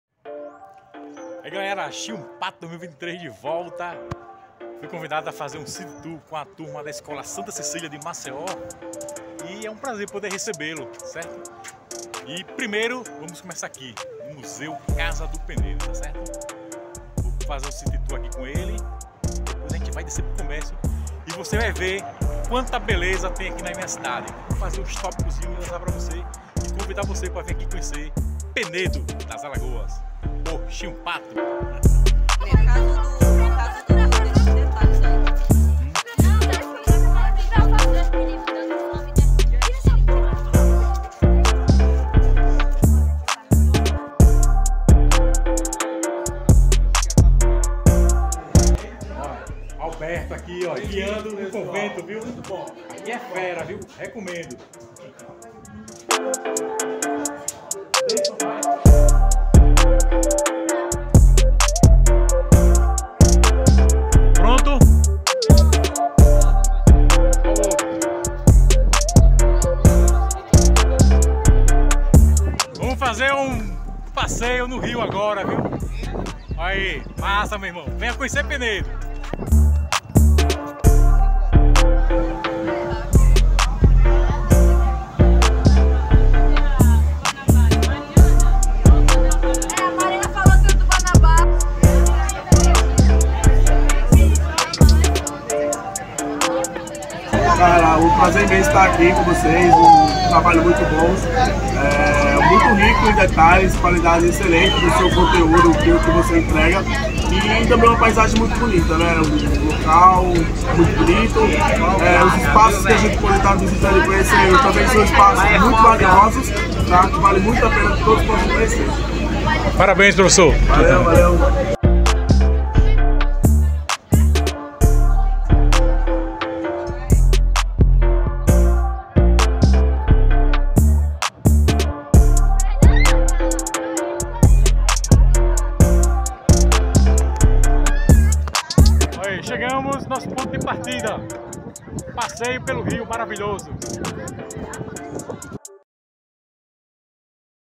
Escola Sta. CECÍLIA de Maceió realizando um CITY-TOUR em PENEDO
Belíssimo passeio por alguns pontos turístico na cidade de Penedo, inclusivo passeio pelo Rio. 22/09/2023